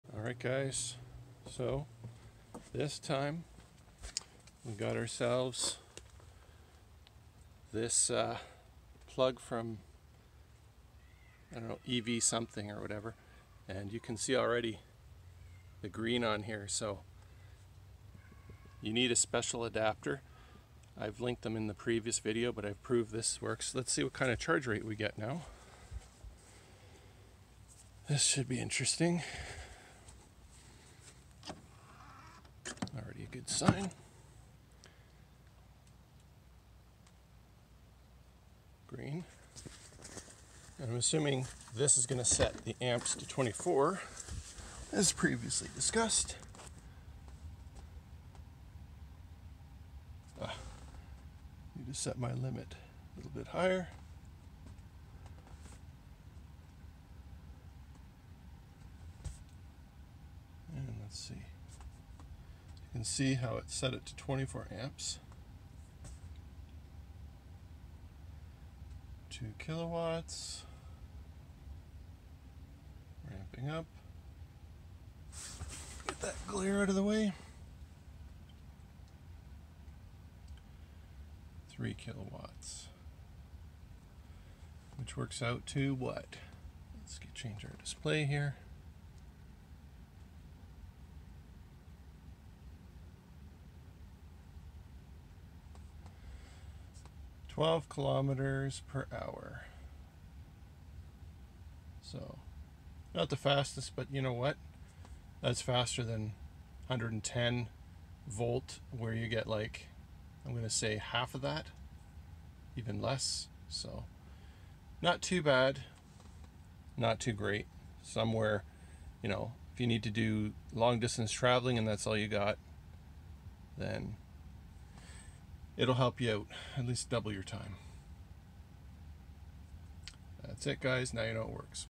Tesla Model 3 | Charging at 30 amp RV Sites? | TT30 plugs - Part 2 (Adapter Tested)
In part 2 of this series where we discuss the TT-30P outlet and charging with your EV, I had 2 options to pick from:

1. TT-30 Adapter for Tesla Model S/X/3/Y Gen 2

2. AC WORKS EV Charging Adapter for Tesla Use (TT-30 RV 30A to Tesla)

3. For our American friends here is a cheaper adapter (like #2 above but cheaper)

I chose the first adapter because it automatically sets the max charge rate to 24 amps, which you would have to MAKE SURE you do manually if you get the second adapter. We can see that I got 3kw @ 24 amps or 12km/hr charge rate and is a rough estimate of what you could expect at these 30 amp RV sites.

Using the Tesla Portable Wall Charger's household 110V adapter you could expect 1kW @ 12 amps or 6km/hr (see example in this video

Another possible solution to improve your speed would be to use a TT-30P splitter to Nema 14-50 (if the campsite has dual plugs or if you have a long extension cord to pull a second line from another plug) which would allow for the faster 50 amp charging rates: